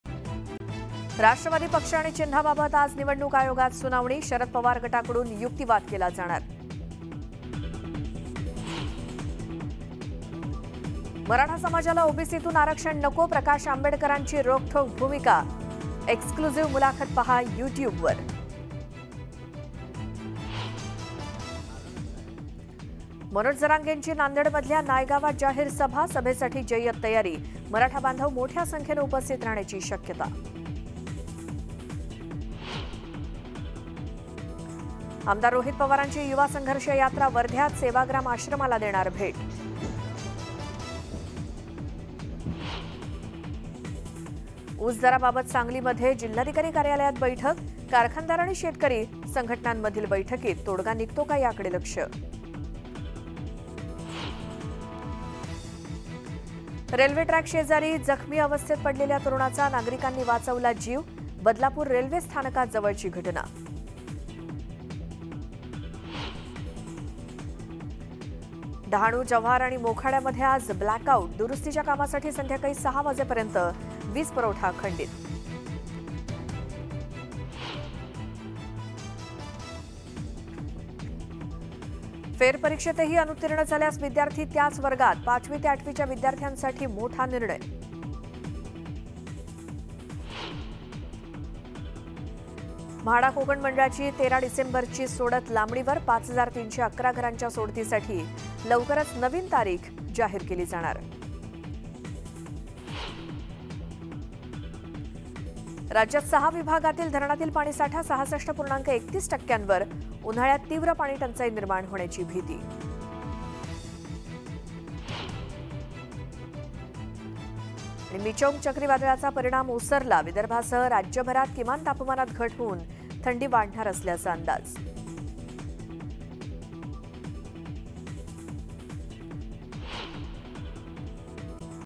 Marathi News Headlines | 7 AM News Today | Latest Maharashtra News | News18 Lokmat | Dec 8, 2023
Second Day of State Legislature Winter Session
Who will get Nationalist Congress, clock symbol? Ajit pawar or Sharad Pawar.
The hearing will be held in the afternoon in the Central Election Commission
MLA Rohit Pawar's 'Yuva Sangharsh Yatra' in Wardhya
Public meeting of Manoj Jarange in Naigaon in Nanded
'Blackout' today in Dahanu, Jhawar and Mokhadiya
Power supply will be interrupted till 6 pm for repair work
Mhada Konkan Mandal December 13 draw postponed

राज्य विधिमंडळ हिवाळी अधिवेशनाचा दुसरा दिवस
राष्ट्रवादी कॉंग्रेस कुणाची, घड्याळ चिन्ह कुणाला?
केंद्रीय निवडणूक आयोगात दुपारी होणार सुनावणी
आमदार रोहित पवारांची 'युवा संघर्ष यात्रा' वर्ध्यात
मनोज जरांगेंची नांदेडमधल्या नायगावात जाहीर सभा
डहाणू, जव्हार आणि मोखाड्यात आज 'ब्लॅकआऊट'
दुरुस्तीच्या कामासाठी संध्याकाळी 6 पर्यंत वीजपुरवठा खंडित राहणार
म्हाडा कोकण मंडळाची 13 डिसेंबरची सोडत लांबणीवर